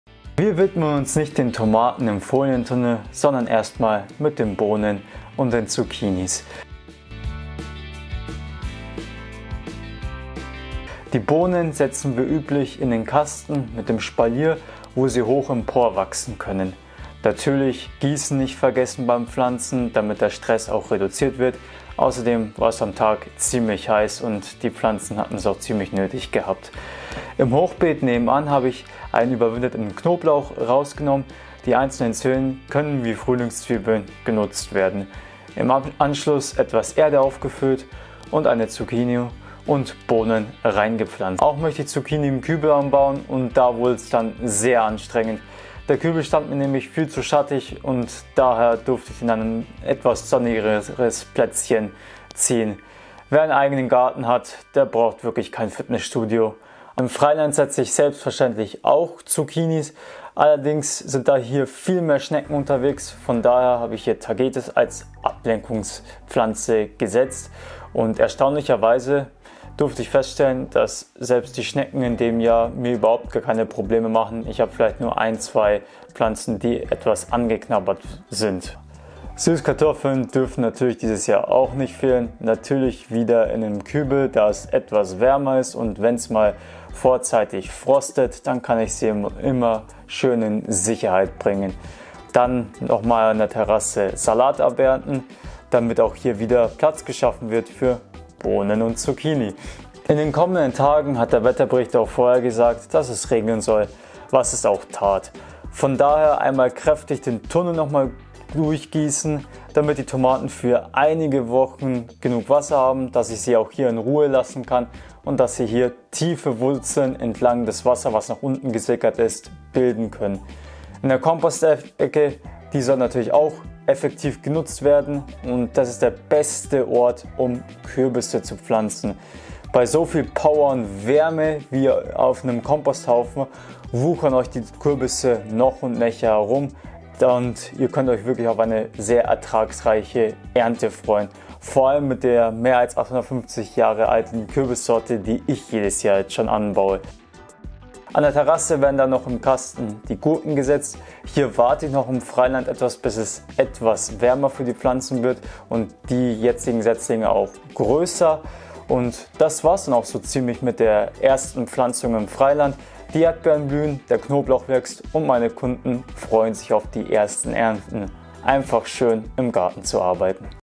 Du wirst Kürbis nur noch so anbauen | Mischkultur Zucchini | Süßkartoffel anbauen - Gartenarbeit Mai
Die Gartenarbeit im Permakultur Projekt ruft und es gibt weitere Freilandauspflanzungen. Ich zeige dir, wo ich meine Kürbisse anbaue für eine reiche Ernte, welche Mischkultur, meiner Meinung nach, die beste zu Zucchini passt und viele mehr.